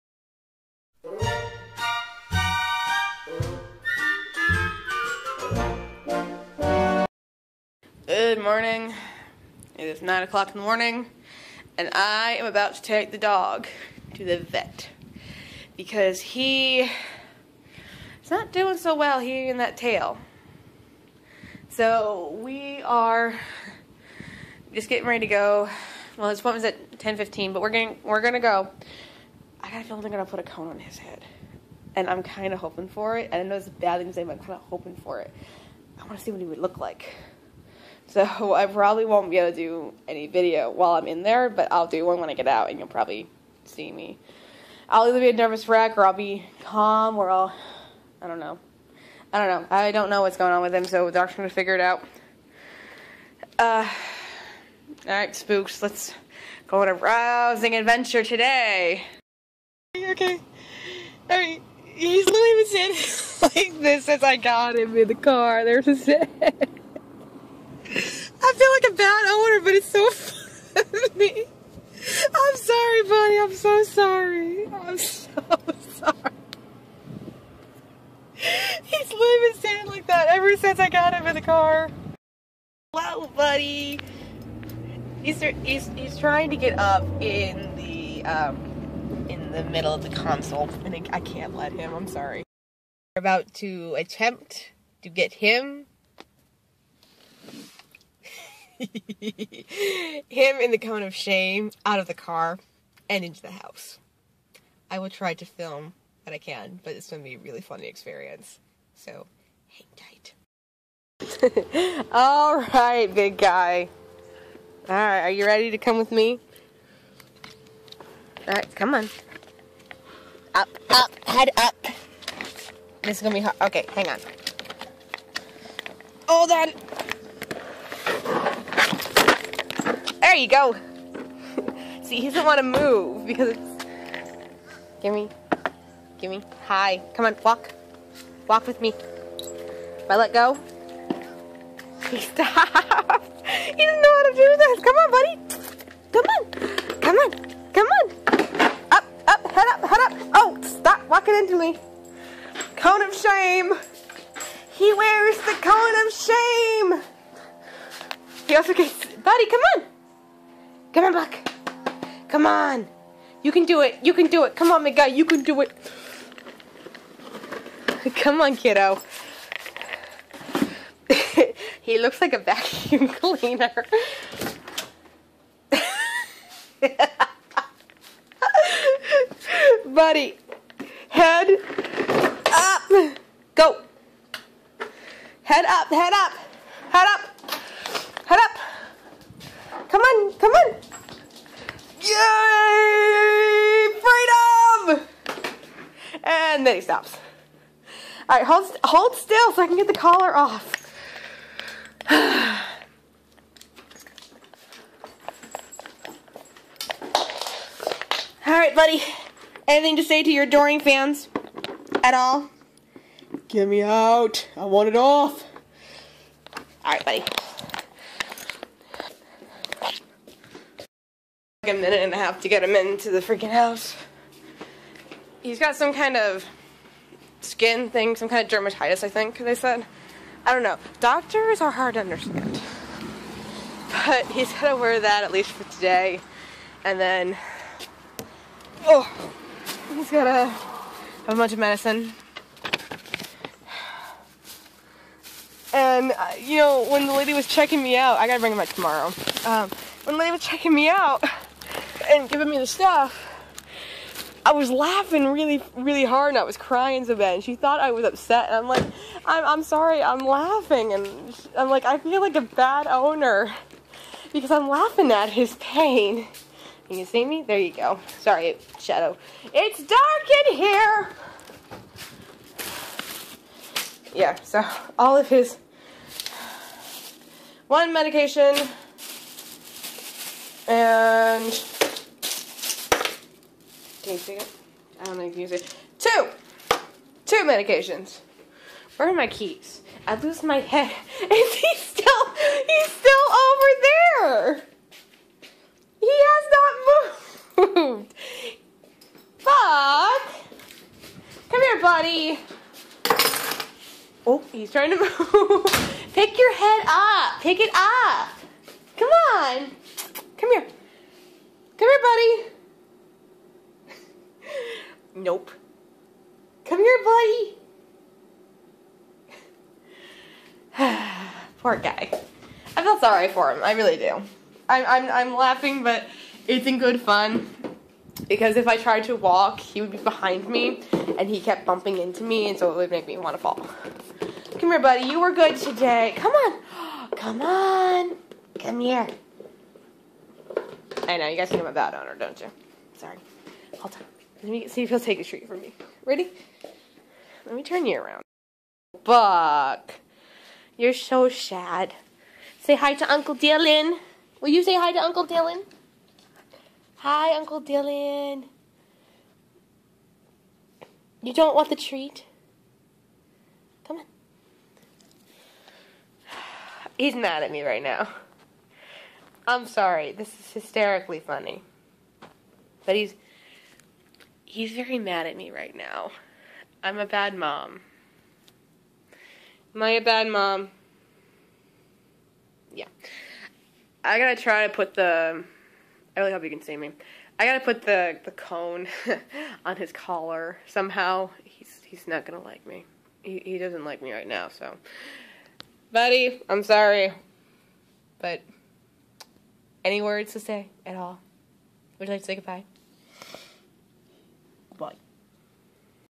The Cone of Shame
Buckshot goes to the vet and I, although a bit heart broken that he is sick, get a laugh at the outcome.